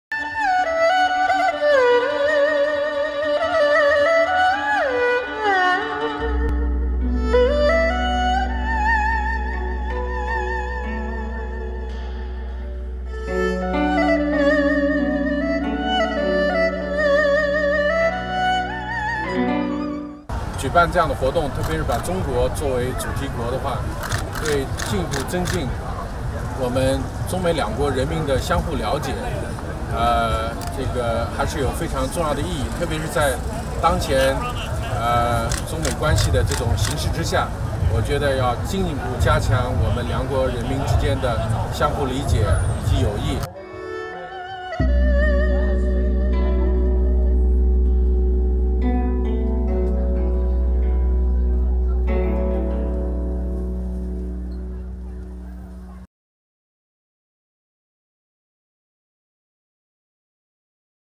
Lotus festival featuring Chinese culture held in LA
The 38th Los Angeles Lotus Festival was held over the weekend. This year it is honoring China and the vibrant Chinese culture which is an integral part of Los Angeles and California.